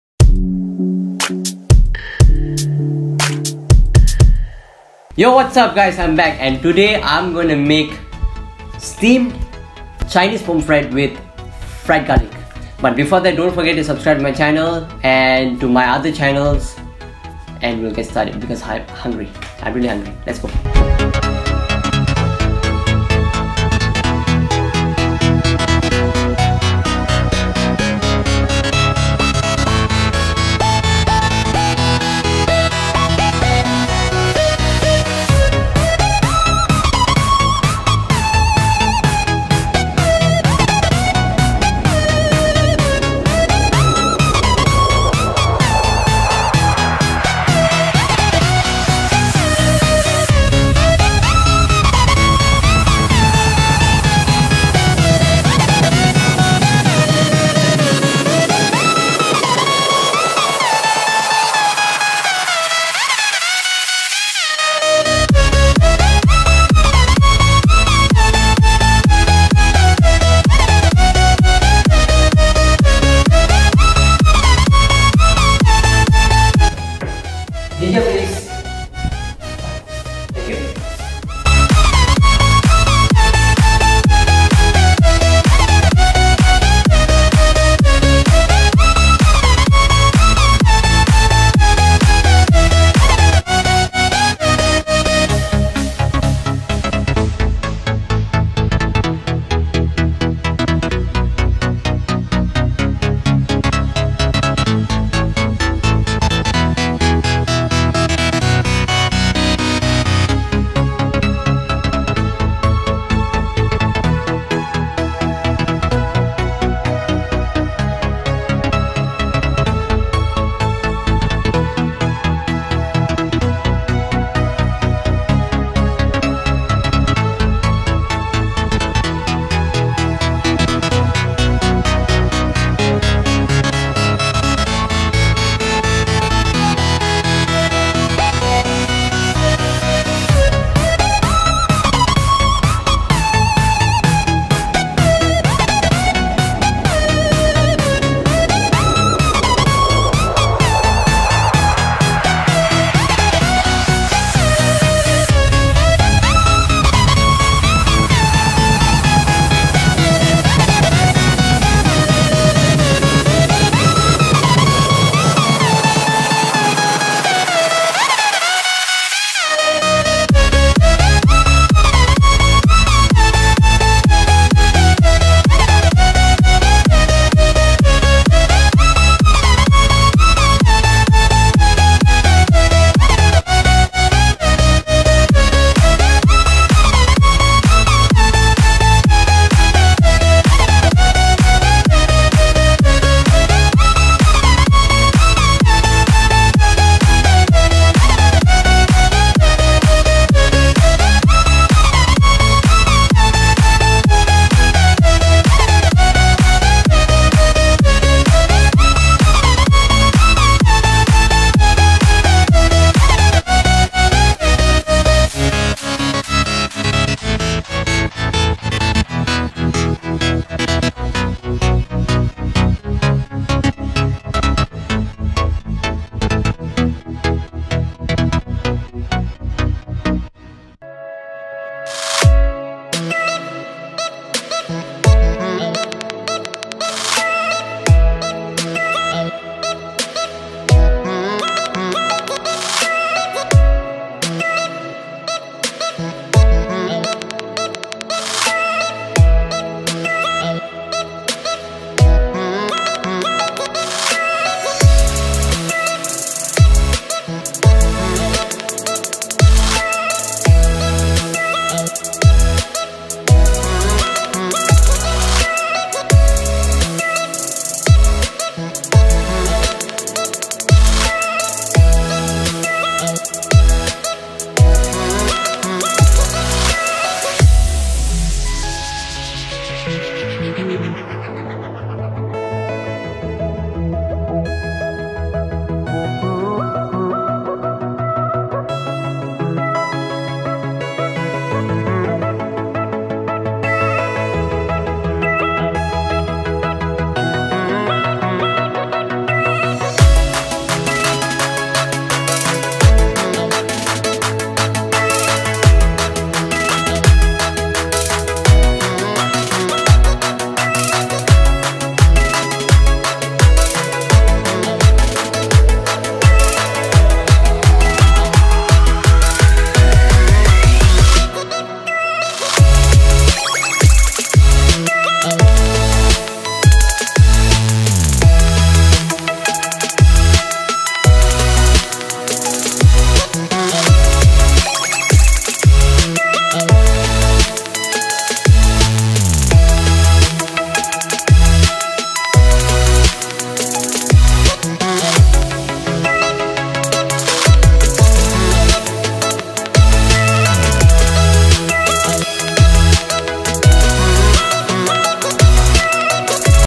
Steamed Chinese Pomfret with Fried Garlic | Easy to make | Syarvin's Kitchen
If you're looking for amazing steamed chinese pomfret with fried garlic, then this is it! Hope you guys love it! It's really easy to make! Good Luck!